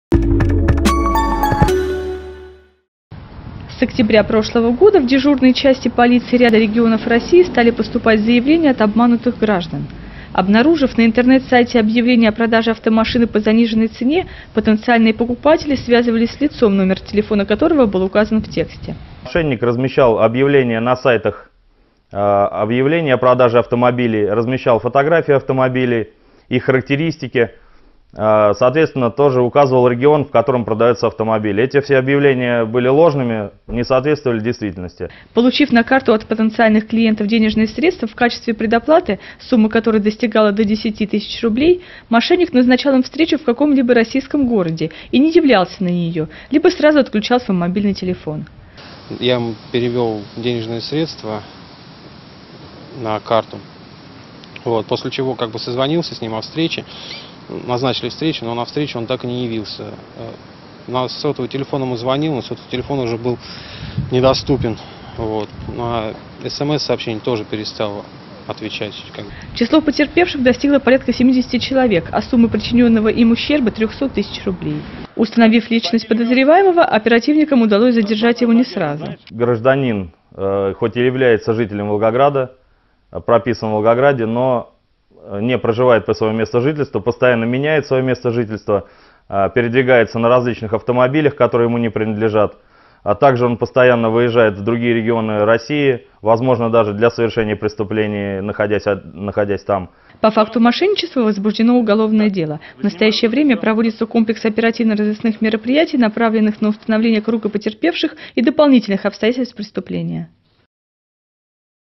Продавал виртуальные авто
Сотрудники отдела «К» ГУ МВД России по Волгоградской области в ходе проведения оперативно-разыскных мероприятий по подозрению в серии мошенничеств с размещением объявлений о продаже автомашин установили личность и задержали 29-летнего жителя Волгогра